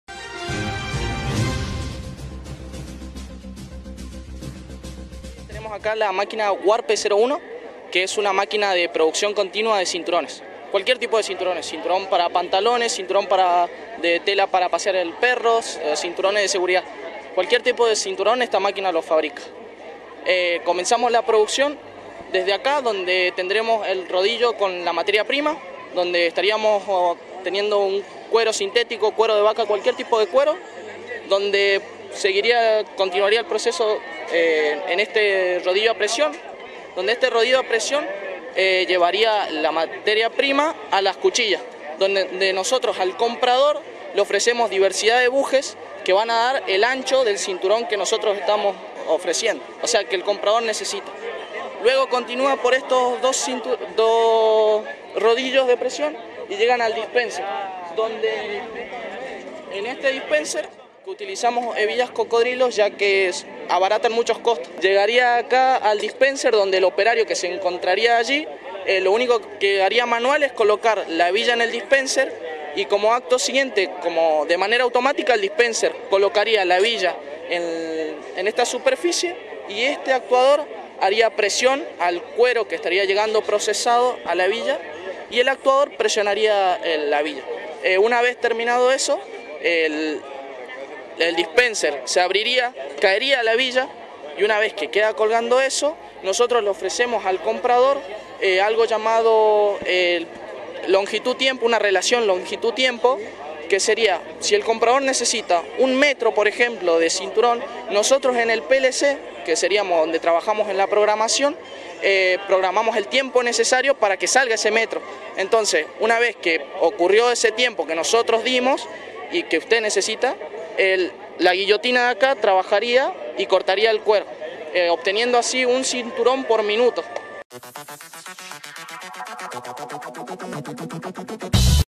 La Escuela Ejército de los Andes presentó su máquina para realizar cinturones
En el marco de la exposición técnica que se realizó el 14 de noviembre en el centro de la ciudad, los chicos presentaron la máquina "Huarpe 01", diseñada especialmente para realizar cualquier tipo de cinturón. Este dispositivo posibilita a través de la materia prima (cuero, telas, etc) pasar por un proceso de idustrialización para crear el cinturón.